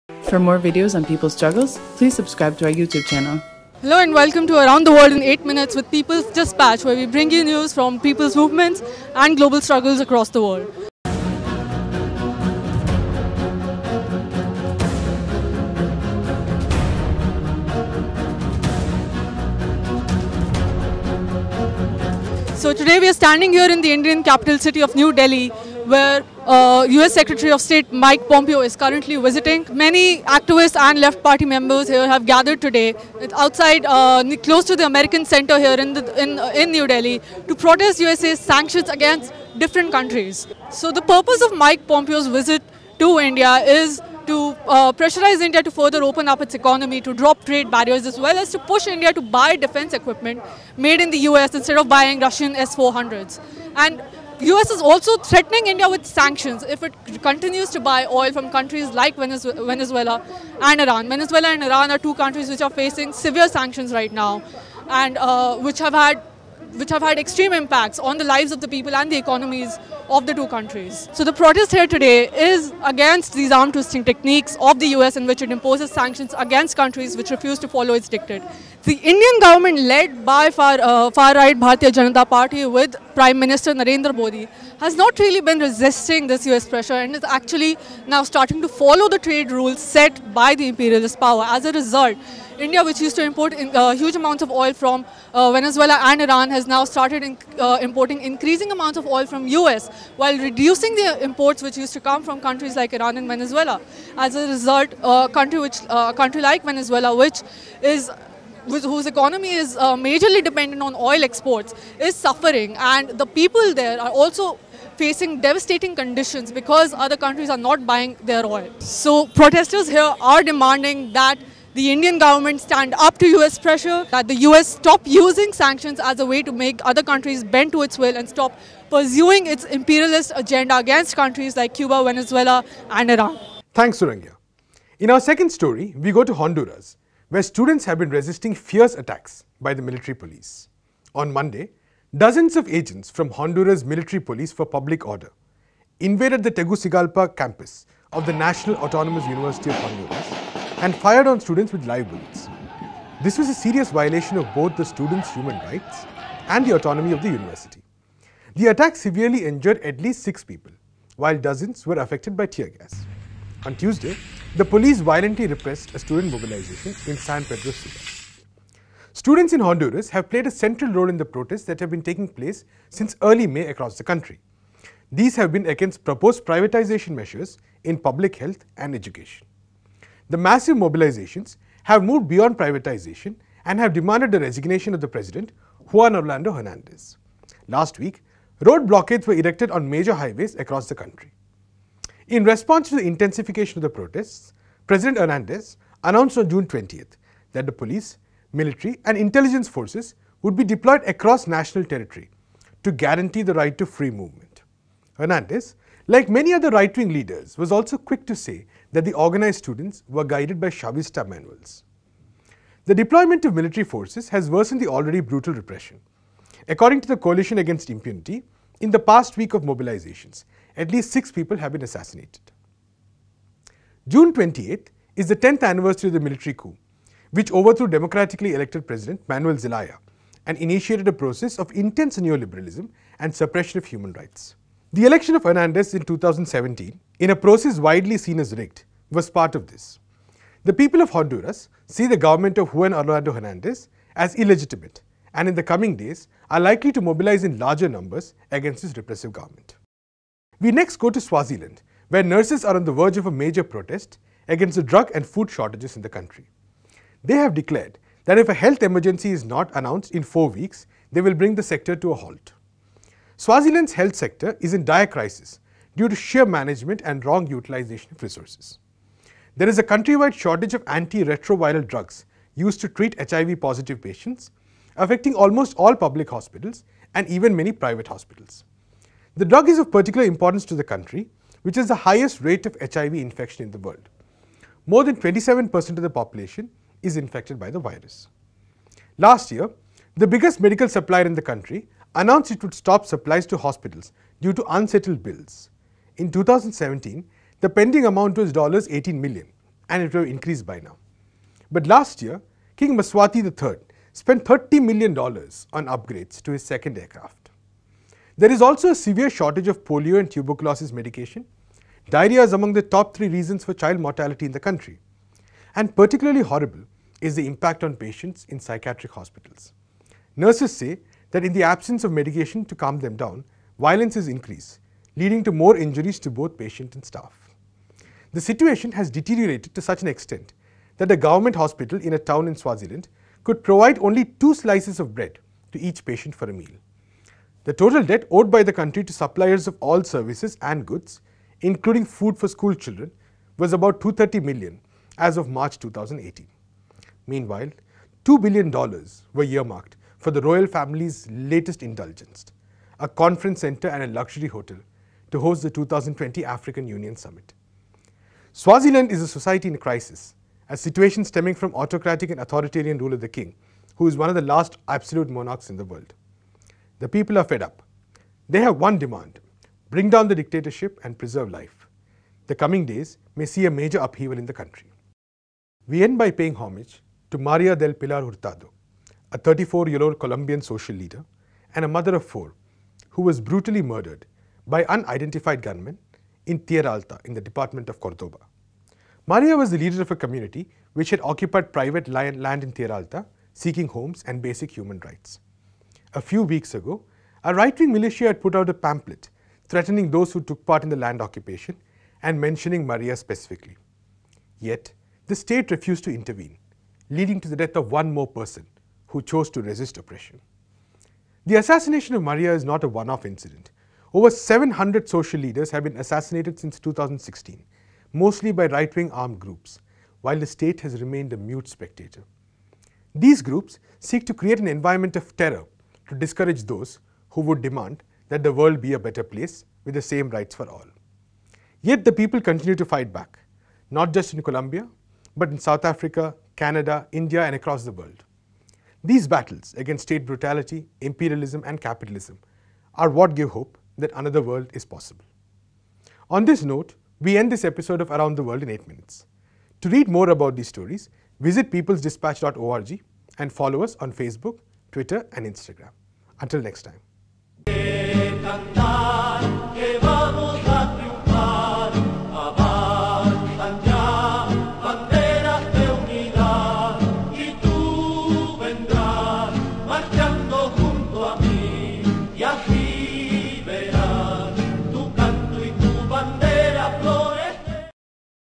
Around the World in 8 Minutes | Episode 32 | International News on Working Class & Popular Struggles
In this episode, we report from the ground in New Delhi on the protest against the visit of US secretary of state Mike Pompeo and US imperialist project in Cuba, Venezuela and Iran. We also look into Honduras where students have been resisting fierce attacks by the military police. In the last story, we repot on Swaziland, where nurses are on the verge of a major protest against the drug and food shortages in the country.